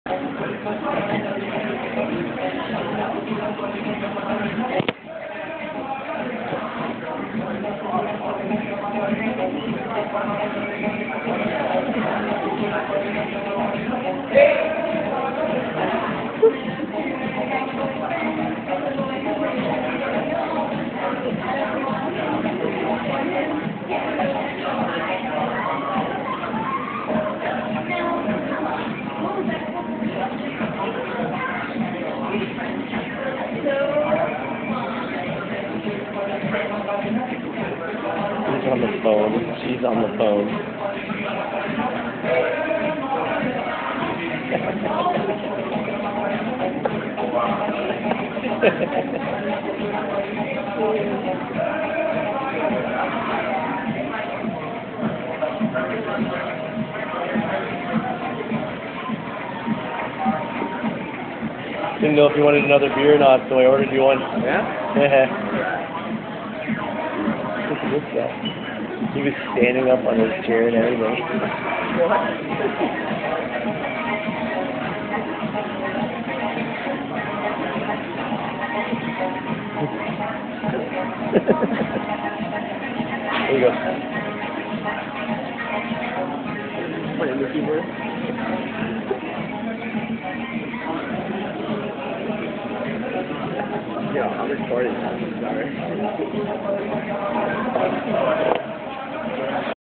Drunk guy dances at Boston Pizza
Drunk guy dances on his bar stool at Boston Pizza in Toronto.